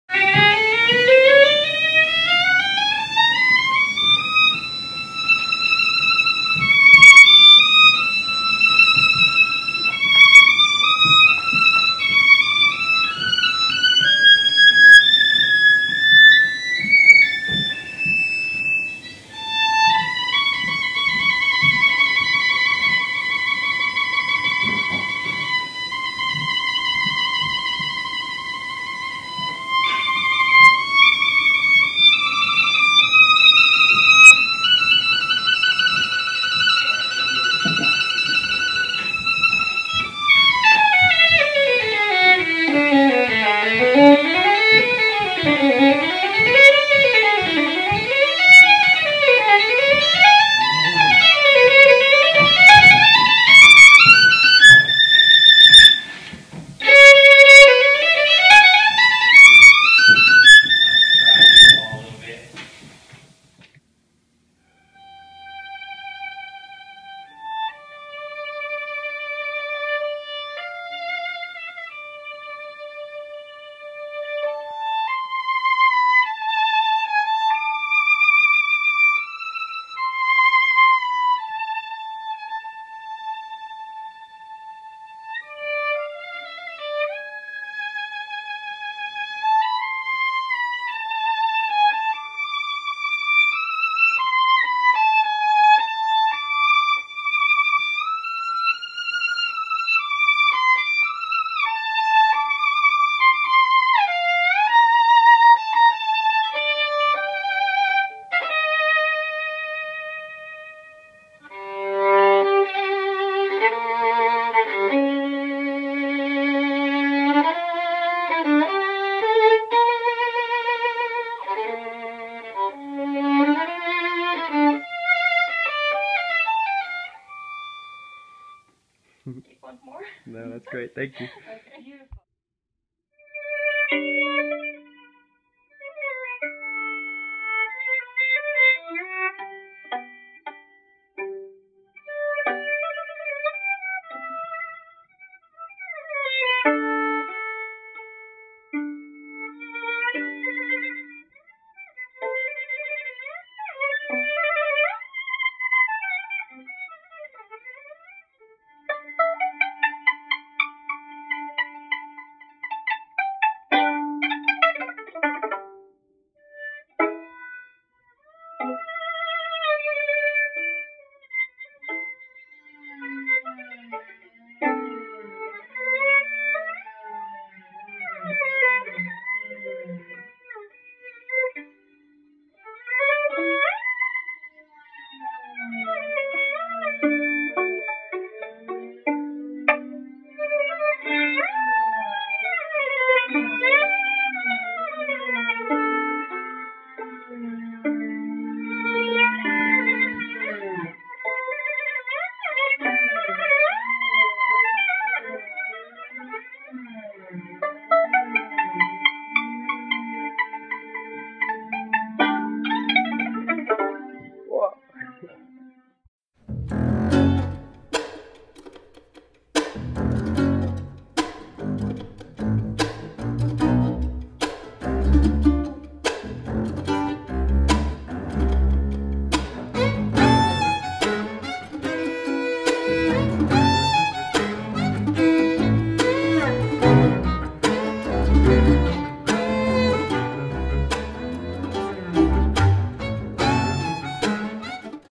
Hostel Violin
3 of our Indy Hostel guests perform a short sample of their craft